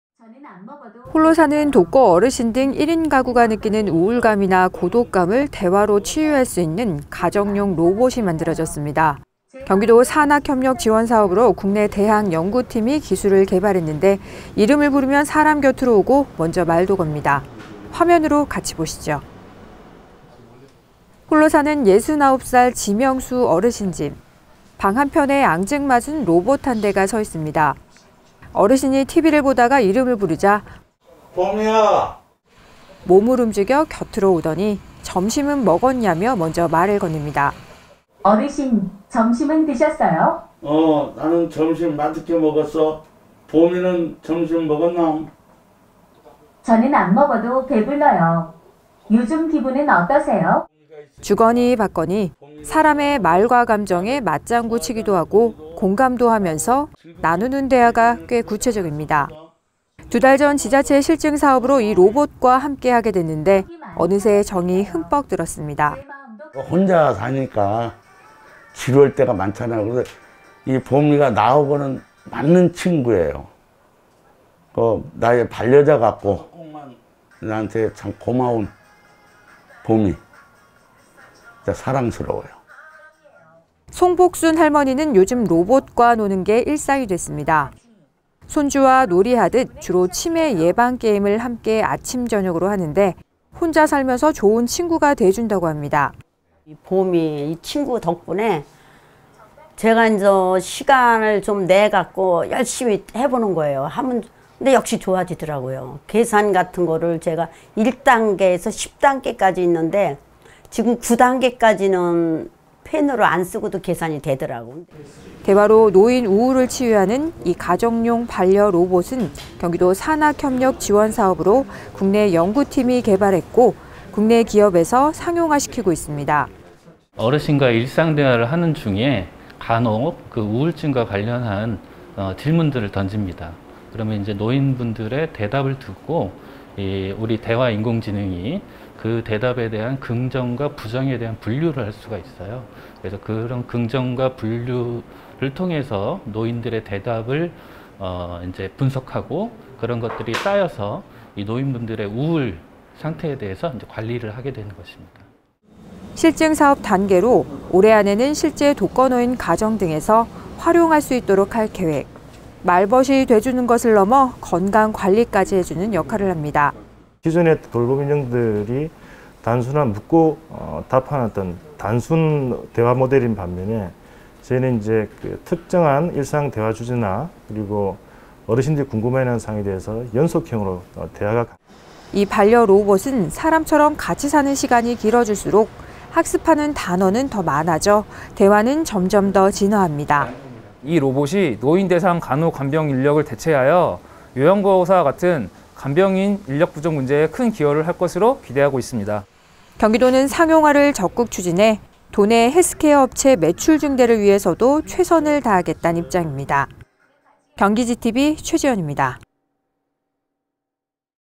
대화로 노인 우울 치유하는 가정용 로봇
[앵커멘트]
홀로 사는 독거 어르신 등 1인 가구가 느끼는 우울감이나 고독감을 대화로 치유할 수 있는 가정용 로봇이 만들어졌습니다. 경기도 산학협력 지원사업으로 국내 대학 연구팀이 개발했는데, 이름을 부르면 사람 곁으로 오고 먼저 말도 겁니다. 화면으로 같이 보시죠.

[리포트]
홀로 사는 예순 아홉살 지명수 어르신 집.
방 한편에 앙증맞은 로봇 한 대가 서 있습니다.

어르신이 TV를 보다가 이름을 부르자 몸을 움직여 곁으로 오더니 점심은 먹었냐며 먼저 말을 건넵니다.
[sync]
어르신 점심은 드셨어요? /어, 나는 점심 맛있게 먹었어. 보미는 점심 먹었나? 저는 안 먹어도 배불러요. 요즘 기분은 어떠세요?

주거니 받거니.
사람의 말과 감정에 맞장구치기도 하고 공감도 하면서.
나누는 대화가 꽤 구체적입니다.

두 달 전 지자체 실증 사업으로 이 로봇과 함께하게 됐는데, 어느새 정이 흠뻑 들었습니다.
[인터뷰] 지명수(69세) 어르신
혼자 사니까 지루할 때가 많잖아요. 그런데 이 보미가 나하고는 맞는 친구예요. 나의 반려자 같고 나한테는 참 고마운 보미, 사랑스러워요.

(화면 전환)송복순 할머니는 요즘 로봇과 노는 게 일상이 됐습니다.
손주와 놀이하듯 주로 치매 예방 게임을 아침저녁으로 하는데, 혼자 살면서 좋은 친구가 돼준다고 합니다.

[인터뷰]송복순(77세) 어르신
보미 이 친구 덕분에 제가 이제 시간을 내 가지고 열심히 (치매 예방 게임을) 해보는 거예요. 하면 역시 좋아지더라고요. 계산 같은 것을 1단계에서 10단계까지 있는데 지금 9단계까지는 펜으로 안 쓰고도 계산이 되더라고요.

대화로 노인 우울을 치유하는 이 가정용 반려로봇은 경기도 산학협력 지원사업으로 국내 연구팀이 개발했고, 국내 기업에서 상용화시키고 있습니다.

[인터뷰] 최진우 /가천대학교 연구교수
어르신과 일상 대화를 하는 중에 간혹 우울증과 관련한 질문들을 던집니다. 그러면 노인분들의 대답을 듣고 대화 인공지능이 그 대답에 대한 긍정과 부정에 대한 분류를 할 수가 있어요. 그런 긍정과 부정의 분류를 통해서 노인들의 대답을 통해서 분석하고 그런 것들이 쌓여서 노인분들의 우울 상태에 대해서 관리를 하게 되는 것입니다.

실증 사업 단계로, 올해 안에는 실제 독거노인 가정 등에서 활용할 수 있도록 할 계획.
말벗이 돼 주는 것을 넘어 건강관리까지 해주는 역할을 합니다.

[인터뷰] 최호영 /로보케어 사업개발팀 부장
기존의 돌봄 인형(로봇)들이 단순한 묻고 답하는 어떤 단순대화모델인 반면에 저희는 이제 특정한 일상 대화 주제나 어르신들이 궁금해하는 현상에 대해서 연속형으로 대화가 가능합니다.

이 반려 로봇은 사람처럼 같이 사는 시간이 길어질수록 학습하는 단어는 더 많아져 대화는 점점 더 진화합니다.

[인터뷰] 안원/경기도 과학기술과 주무관
이 로봇이 노인 대상 간호 간병 인력을 대체하여 요양보호사와 같은 간병인 인력 부족 문제에 큰 기여를 할 것으로 기대하고 있습니다.

경기도는 상용화를 적극 추진해 도내 헬스케어 업체 매출 증대를 위해서도 최선을 다하겠다는 입장입니다. 경기GTV 최지현입니다.

영상취재: 김현우, 영상편집: 윤지성

[자막]

1. 광명시 광명동

2. [sync]

3. [인터뷰] 지명수(69세) 어르신

4. [인터뷰]송복순(77세) 어르신

5. 경기도 산학협력 지원으로 정신건강 관리 인공지능 헬스케어 서비스 개발…상용화 코앞

6. [인터뷰] 최진우 /가천대학교 연구교수

7. [인터뷰] 최호영 /로보케어 사업개발팀 부장

8. [인터뷰] 안원/경기도 과학기술과 주무관

9. 영상취재: 김현우, 영상편집: 윤지성